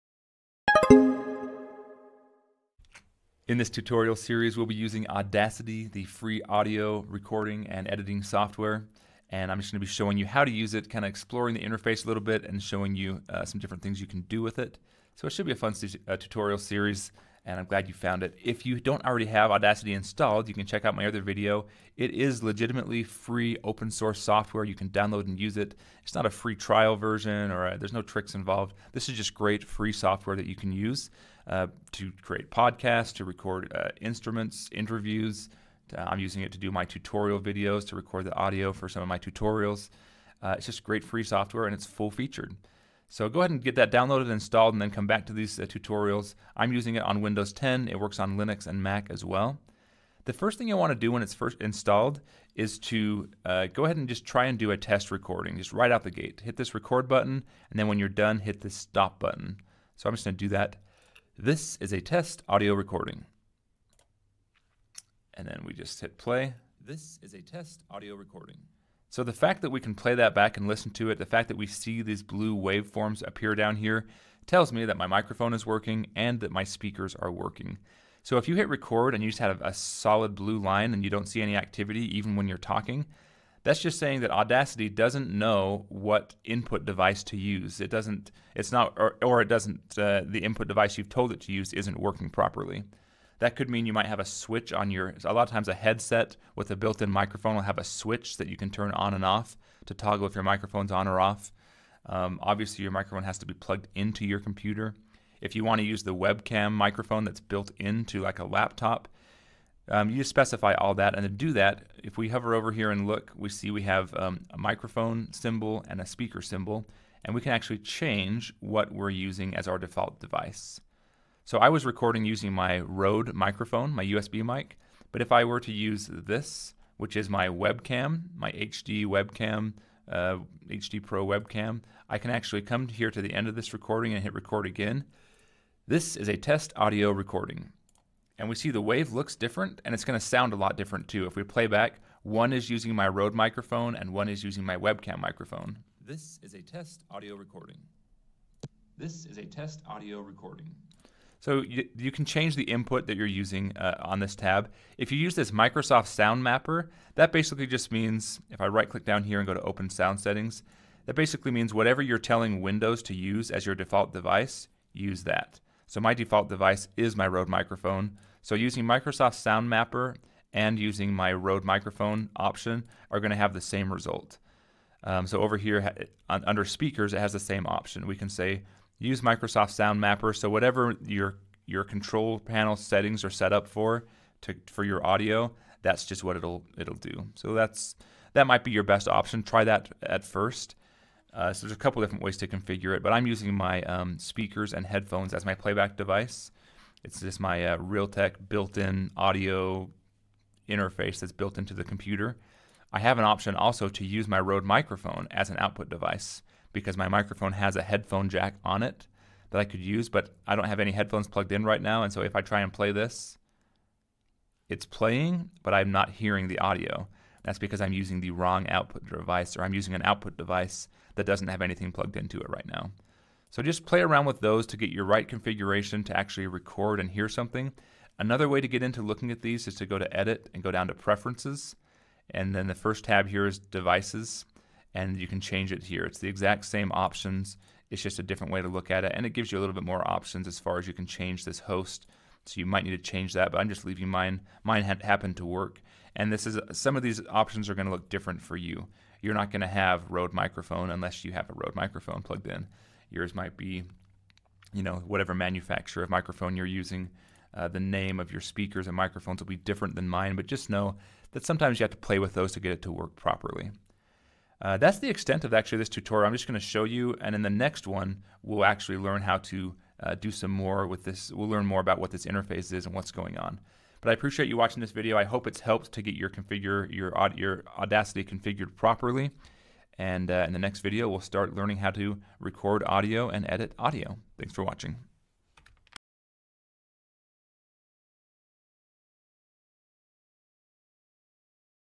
Audacity Lesson 1 - Record, Play, Input, Output
Audacity is a simple, but powerful, free audio recording and editing program for Windows, Mac, and Linux.